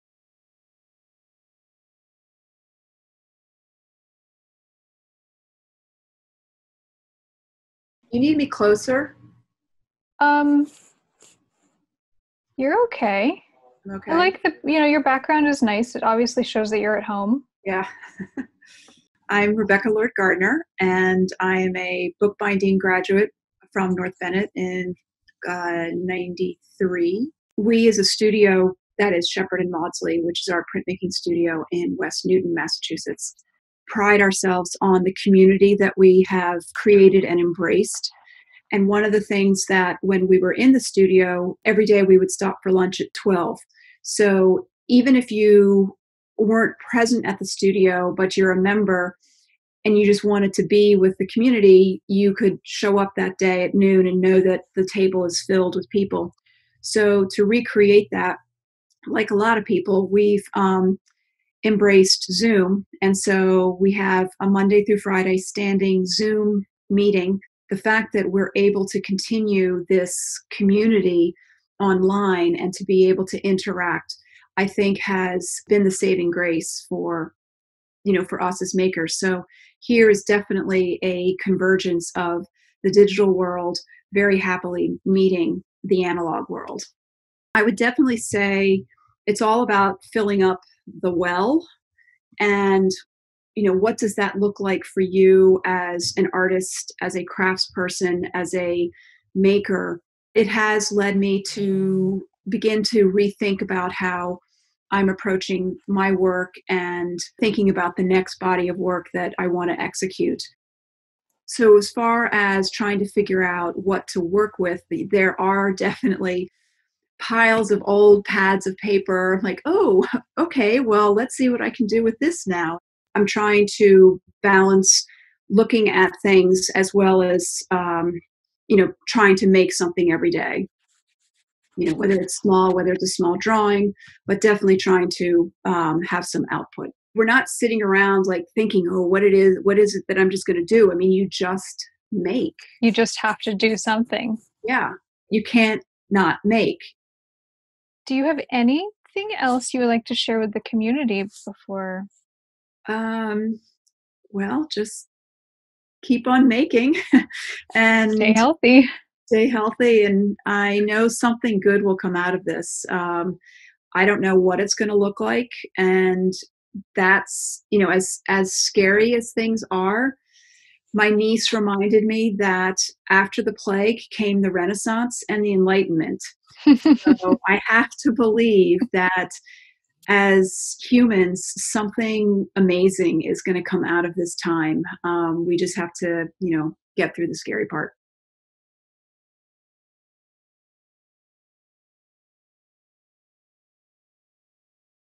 In the Making: Rebekah Lord Gardiner BB '93
Rebekah Lord Gardiner BB '93 is an NBSS Bookbinding graduate, a member of the School's Board of Advisors, and co-owner of Shepard & Maudsleigh Studios, a 25-member printmaking studio / artists space in West Newton, MA. In this interview with Rebekah, we discuss creating community online, the parallels between makers and scientists, and how, when things may seem bleak in the world right now, it's also a moment ripe with creative possibility.

"In the Making" is a series of video interviews with members of our community where we get insight to their lives and work.